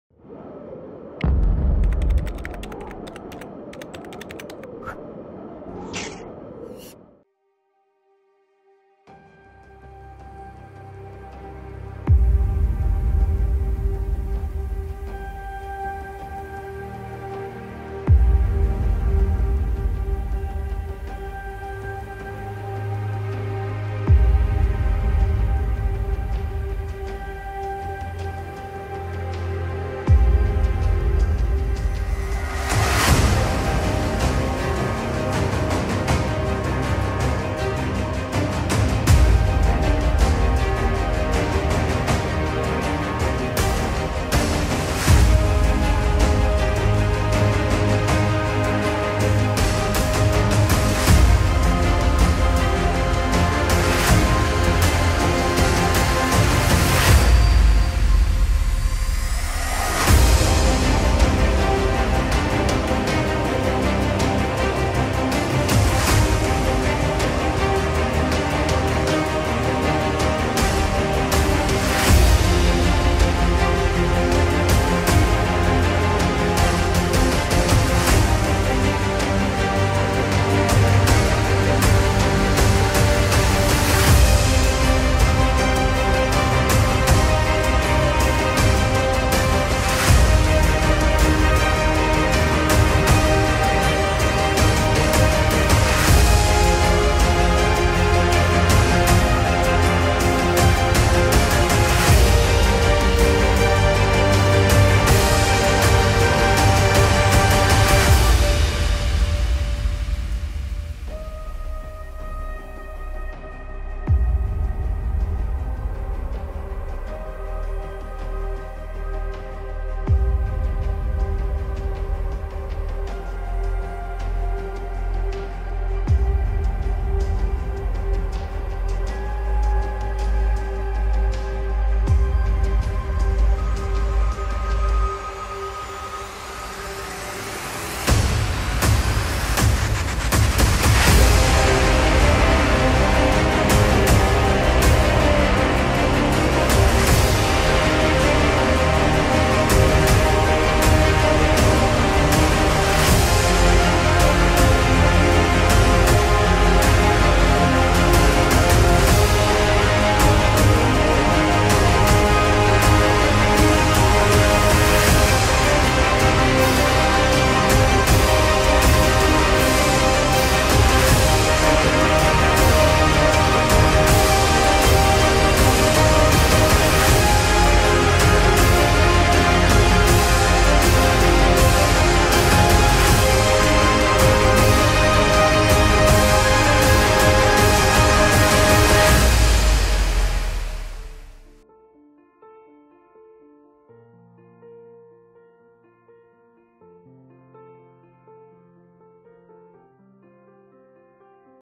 SOARING THE ALPS - Gliding 2022
Gliding season 2022 in the French southern and northern Alps with the Ventus HQ.